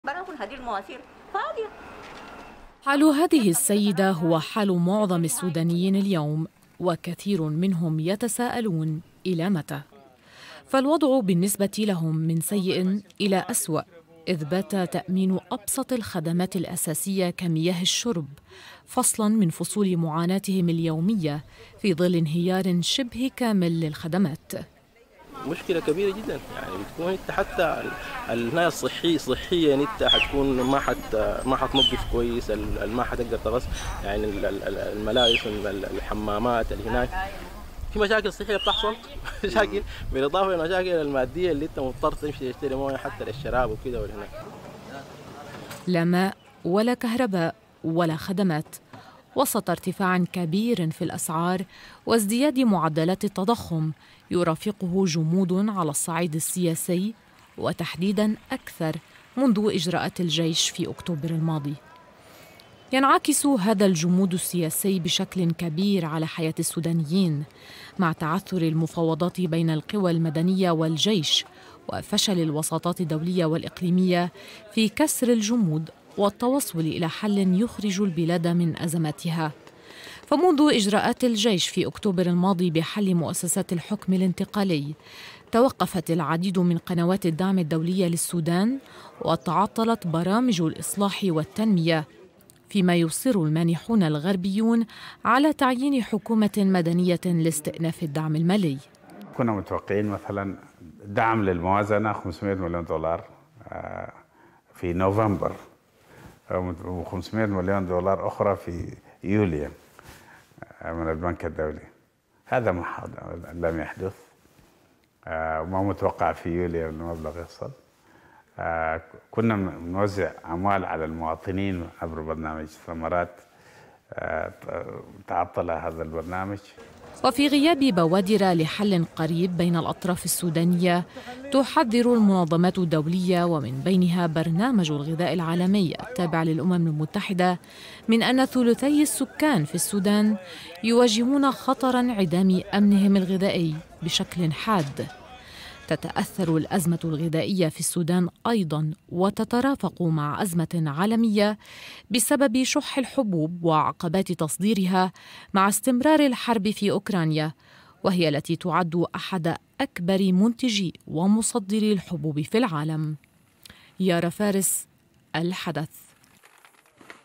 معاناة السودانيين المعيشية تتفاقم.. "حالنا يرثى له"
من سيء إلى أسواء.. السودانيون يعانون الأمرين "أوضاع معيشية صعبة وجمود سياسي مستمر"